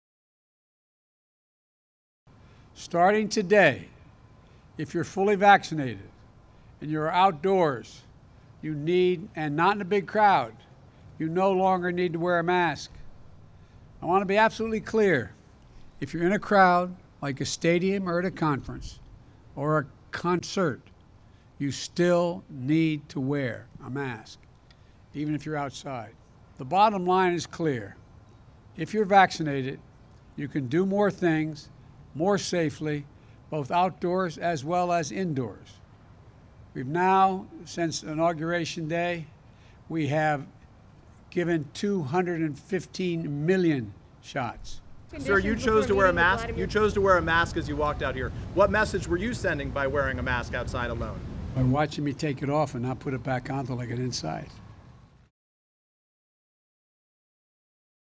Vaccinated can go maskless outside, except in crowds | AJ
“Watch me take it off and not put it back on until I get inside.” US President Joe Biden explains new COVID-19 guidelines on face masks for fully vaccinated Americans.
Thumbnail: New mask guidance for vaccinated Americans